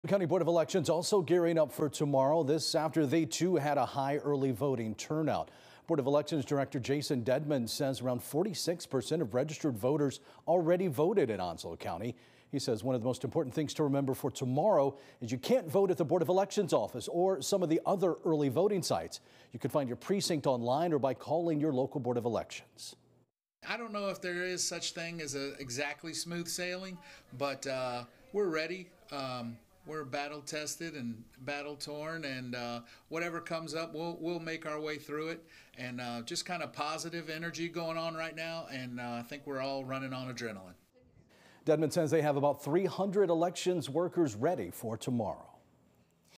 Onslow County Board of Elections gears up for Election Day
The Onslow County Board of Elections is gearing up for Election Day Tuesday.